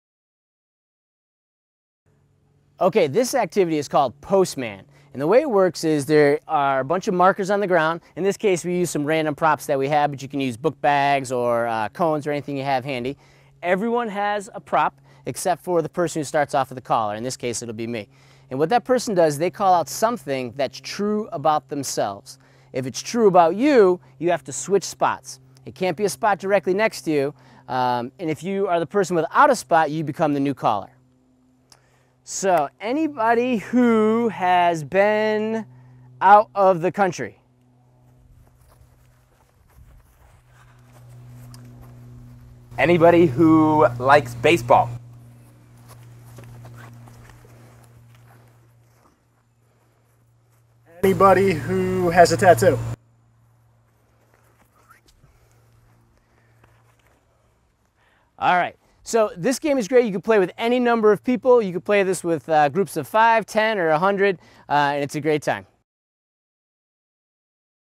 Ice Breakers - Postman - Ultimate Camp Resource
Postman is a fun ice breaker, and the perfect way to help your group learn more about each other.  Find the written description of this activity and more like it, as well as more than 1,000 camp related games, skits, songs, and other activities at UltimateCampResource.com.

Filmed at YMCA Camp Mataucha in Watertown, CT by real life camp staff.